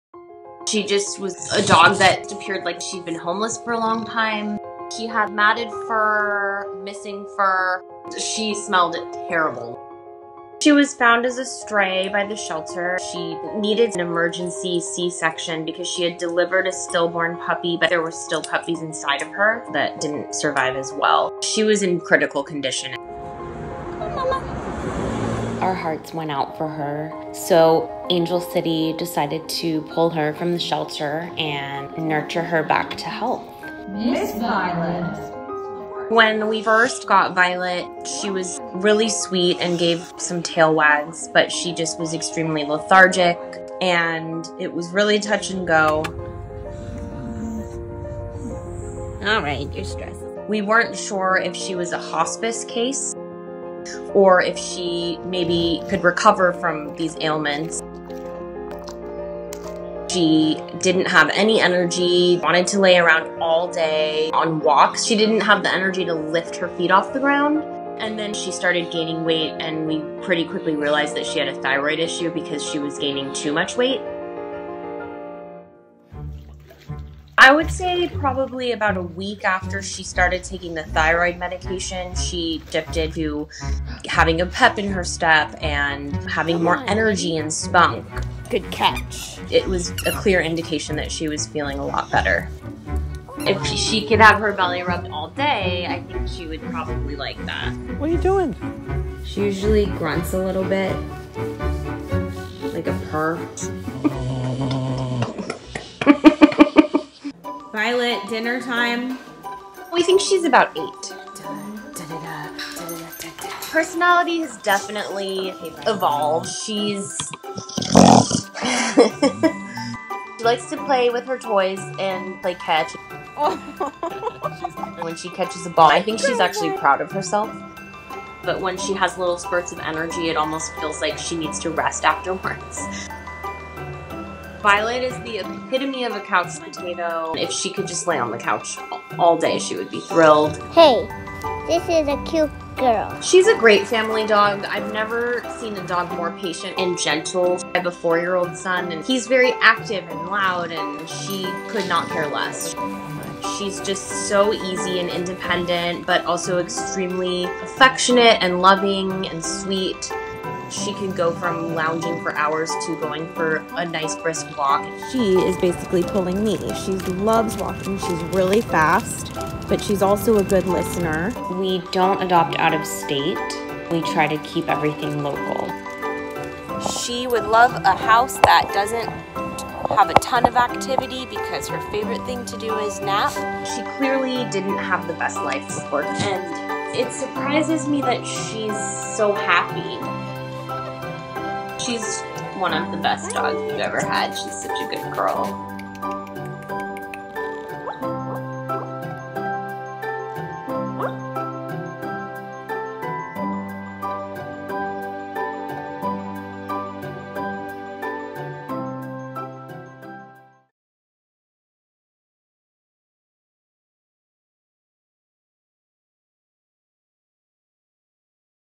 Perrita callejera se convierte en la mejor compañera de abrazos | ¡Adóptame! | El Dodo
Violet, la perrita pitbull, estaba tan triste cuando fue rescatada porque había perdido a sus perritos, pero conoció a su familia de paso y dejó a relucir la personalidad más adorable  ❤️

Para más rescates de Angel City Pit Bulls puedes seguirlos en Instagram: thedo.do/angelcitypits.
Un agradecimiento especial a Rita Earl Photography, puedes ver su trabajo en Instagram: thedo.do/ritaearlphotography.